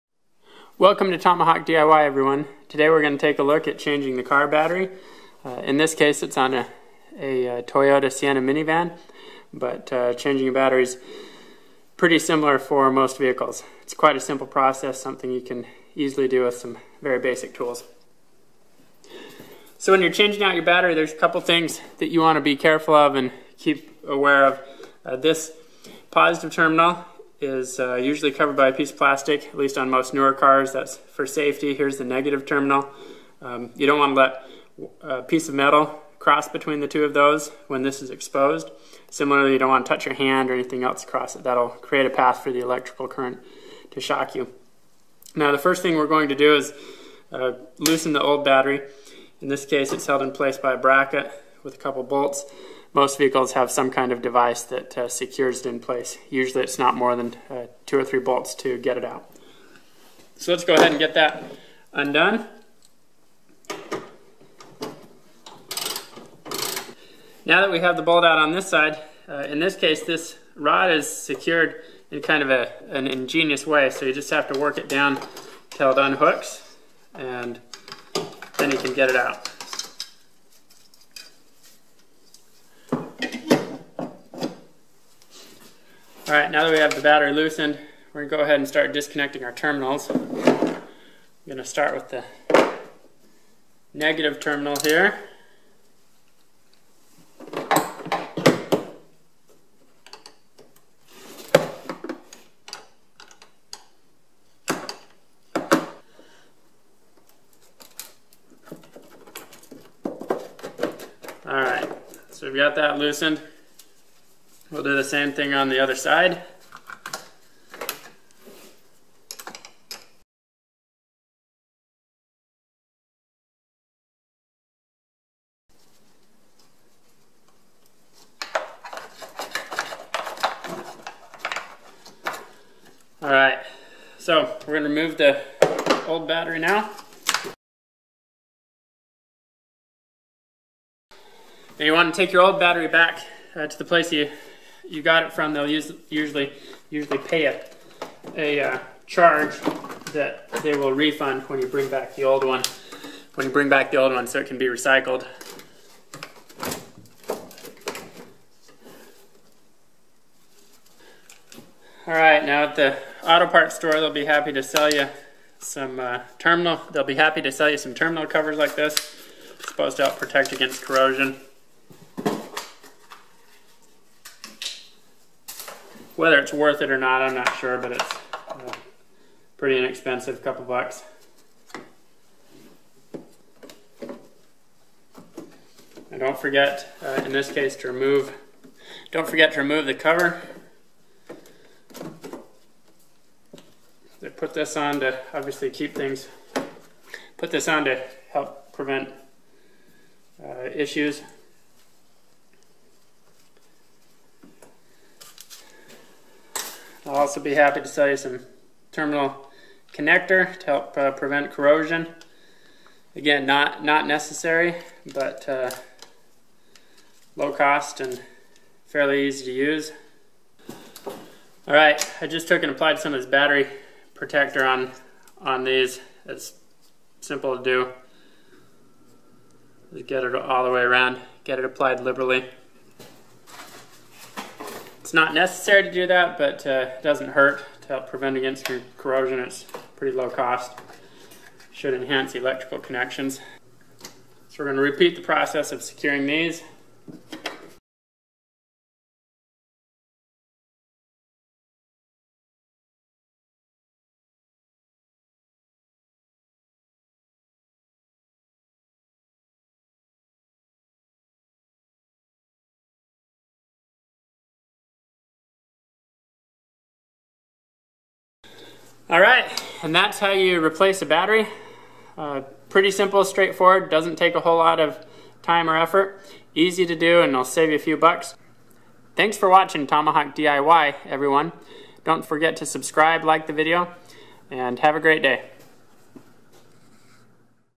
How to Replace a Car Battery (or Van / Truck)
Did your car, truck, or van battery die?  It's easy to replace a battery for your vehicle.  This video will show you how!